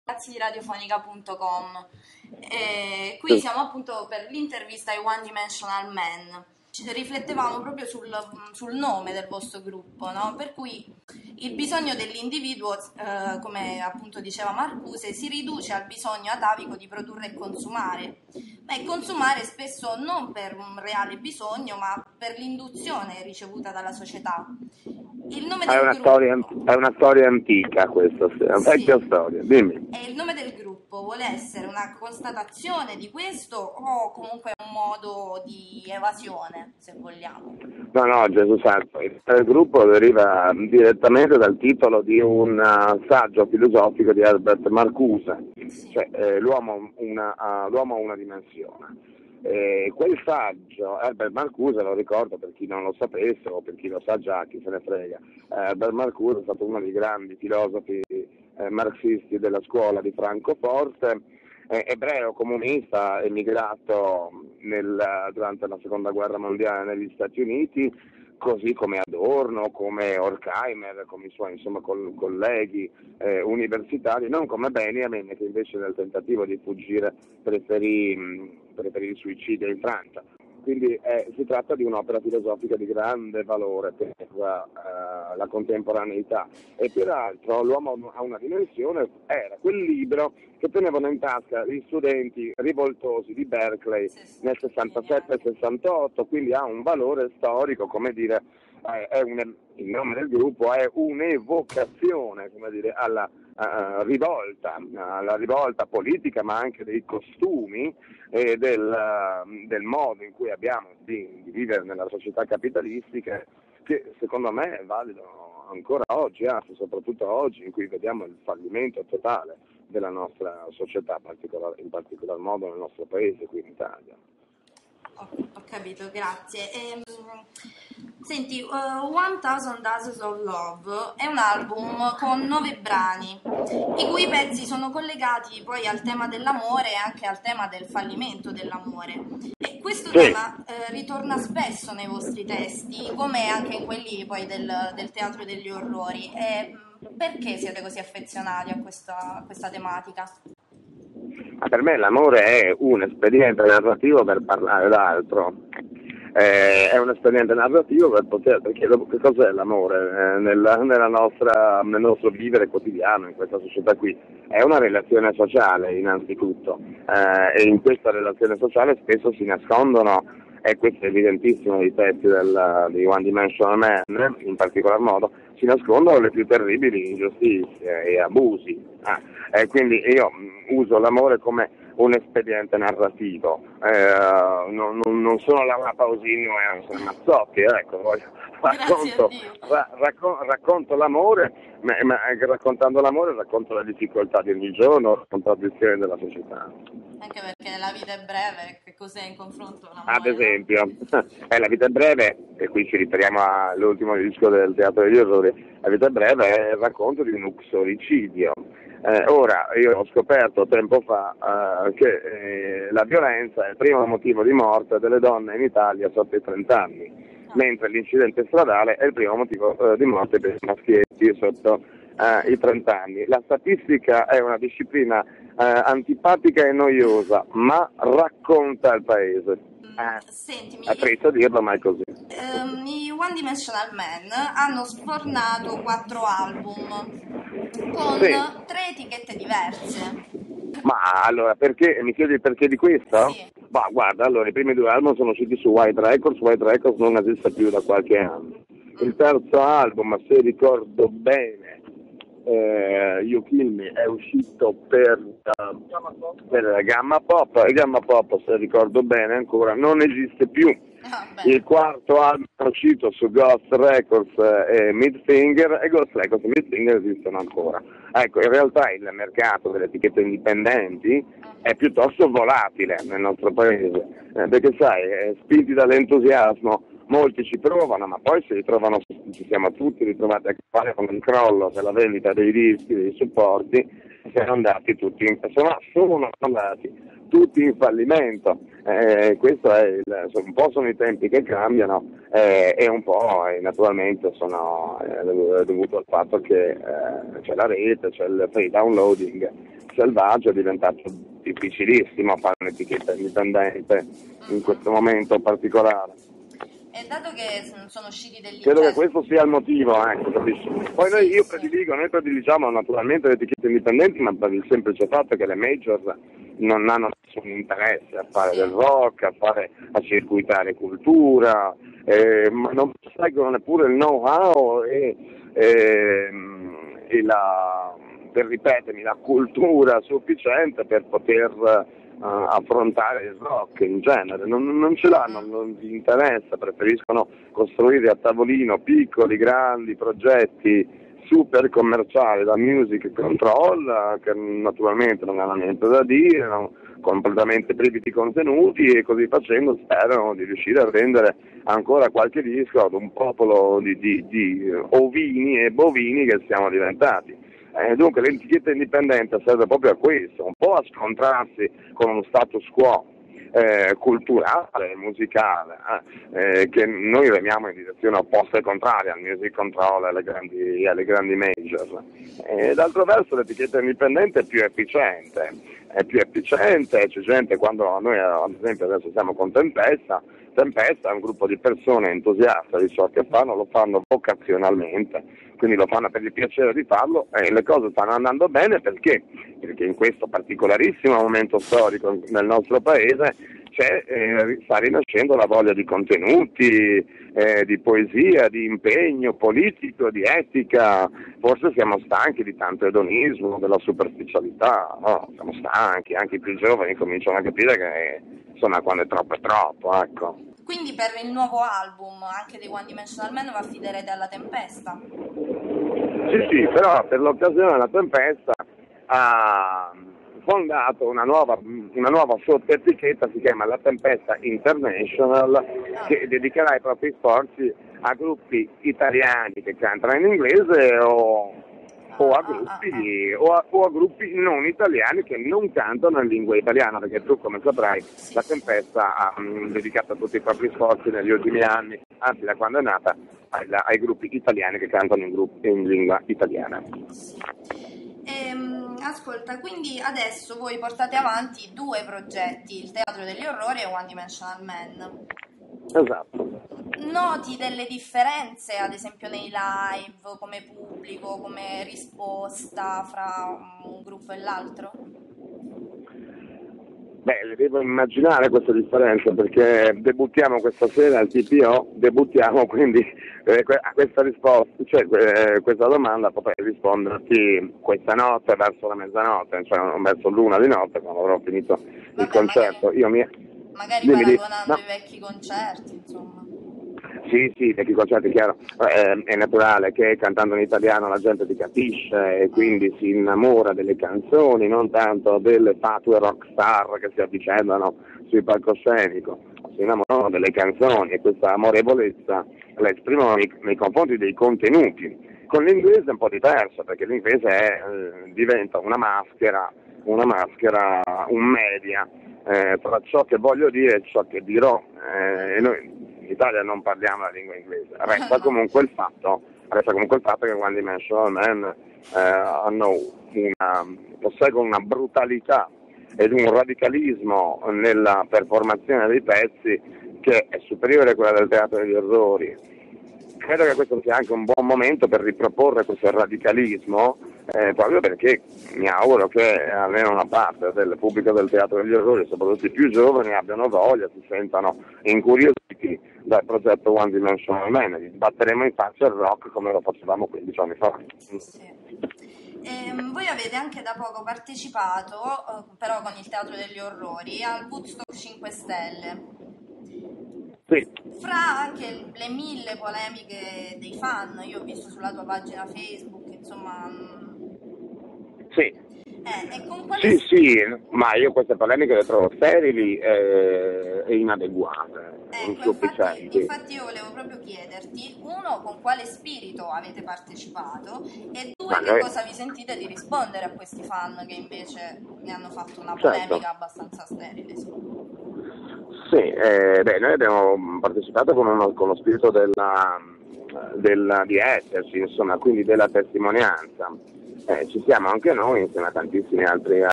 ONE DIMENSIONAL MAN 1a parte intervista - RADIOPHONICA.COM
Prima parte dell'intervista telefonica realizzata con Pierpaolo Capovilla degli One Dimensional Man